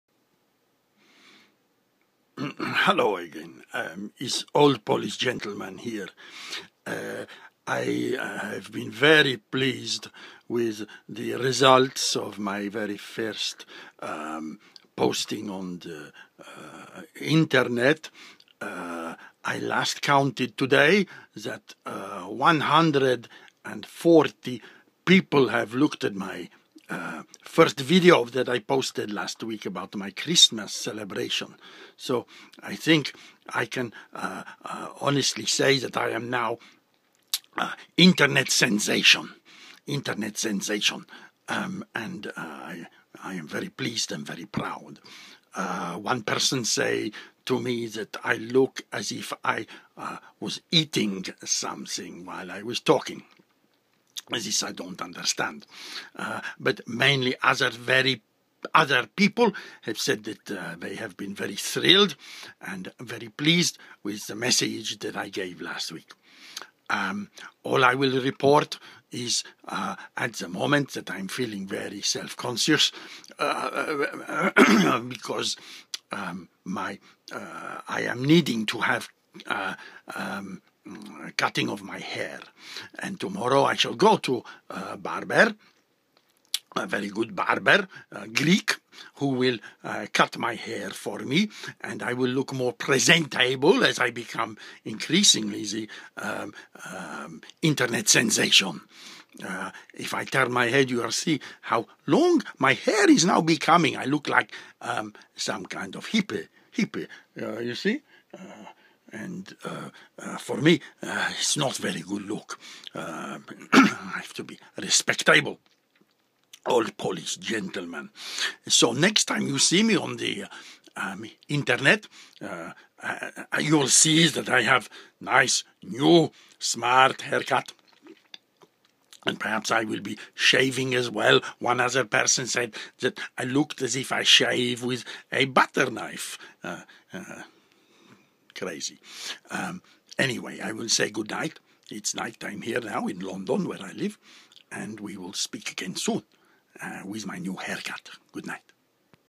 Old Polish Gentleman needs a haircut
Internet sensation & visit to the barbers